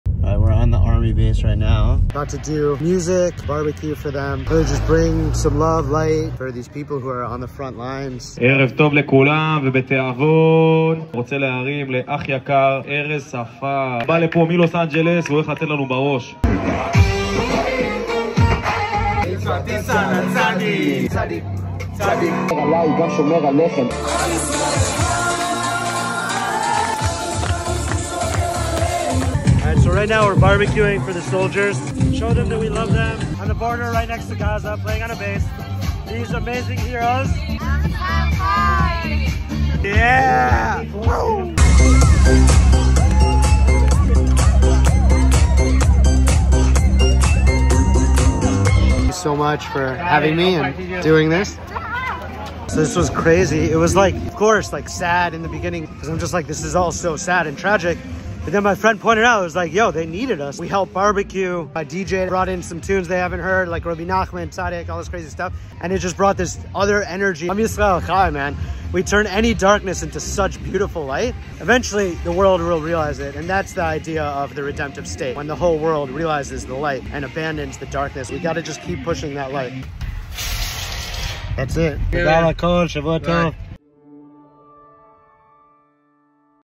I went to the border of Israel and Gaza. This is what happened.
I went to the border of Gaza to bbq and DJ for our brave soldiers, here’s a little taste of the elevated vibes⚡️Am Yisrael Chai!🪬🤍🇮🇱🕊️

Much love,
- Erez Safar🕊